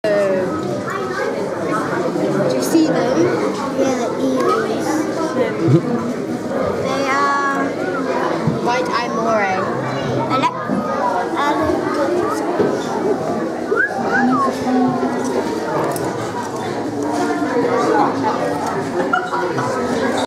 White eye moray eel (gymnothorax thyrsoideus) at london zoo
White eye moray (gymnothorax thyrsoideus) also known as Greyface Moray, freckled moray, slender moray and white-eyed moray, is a moray eel found in coral reefs in the Pacific and Indian Oceans.

If you enjoy reading about strange and weird animals you may want to check my strange animals site: